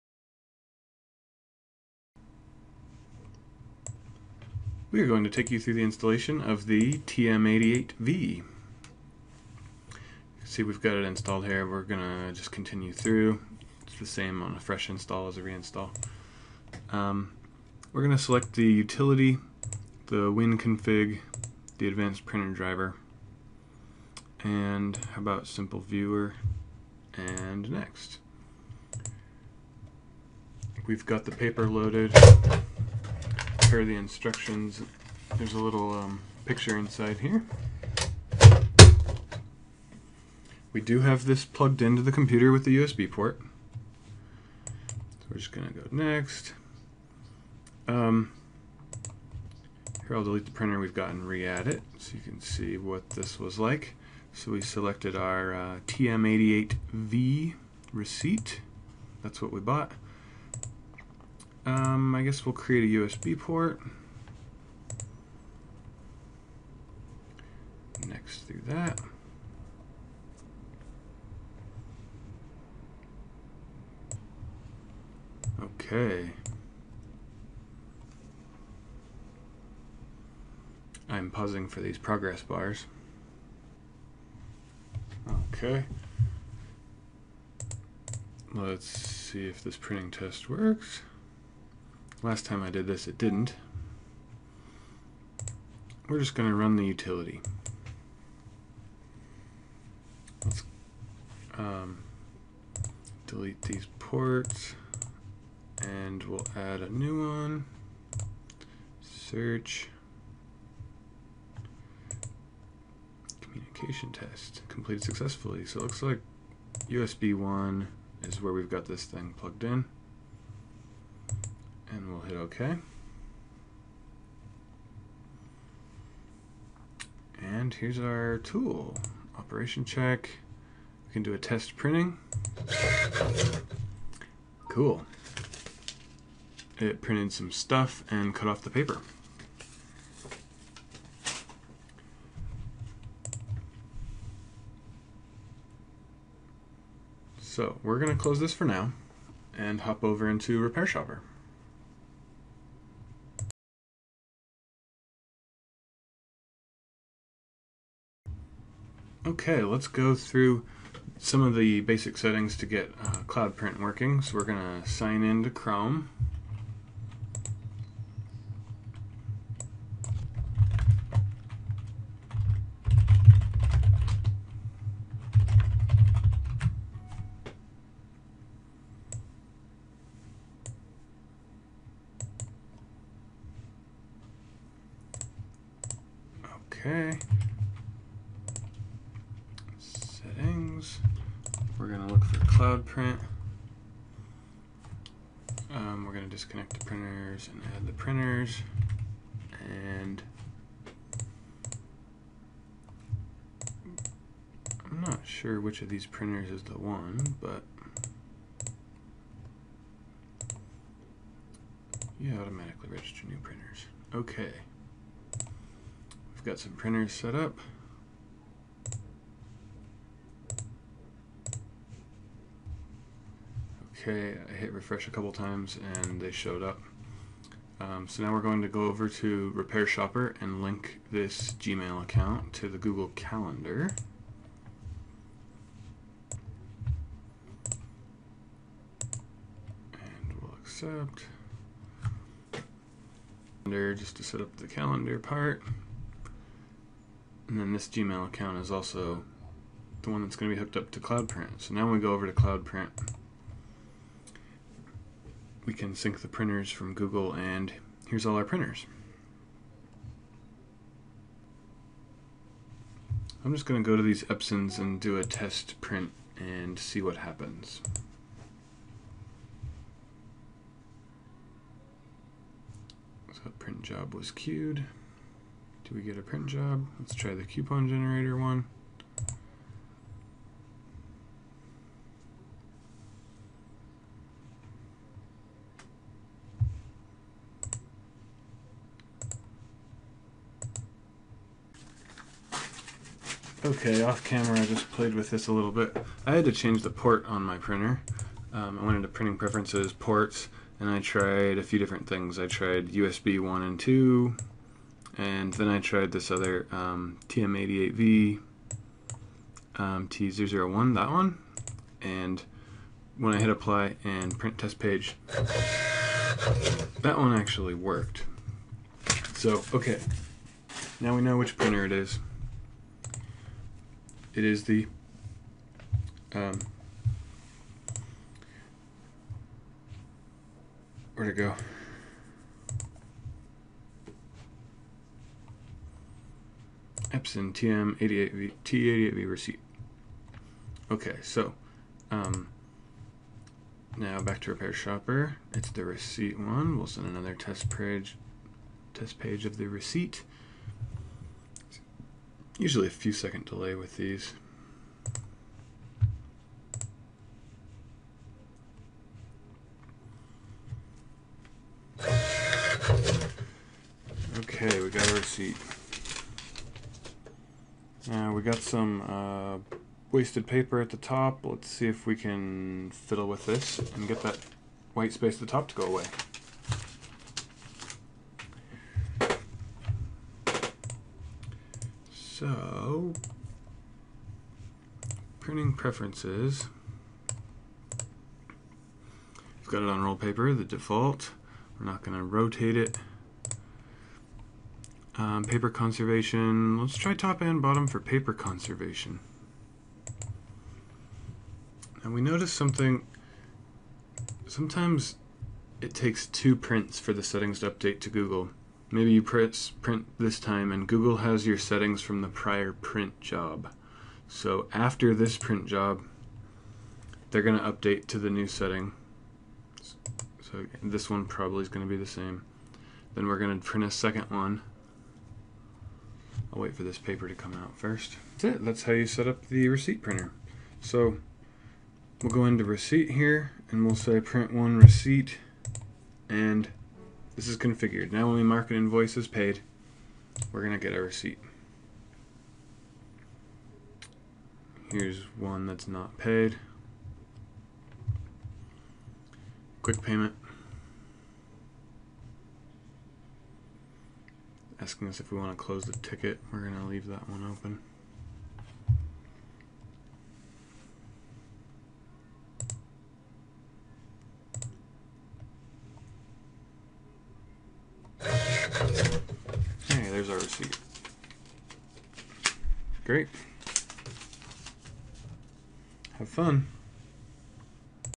RepairShopr - Setup a receipt printer in Windows + Cloud Print
Here in this (somewhat long) video we take you through installing an Epson TM88V receipt printer, adding it to Google Cloud Print, connecting it to RepairShopr, and adjusting the print settings so that it looks good! whew!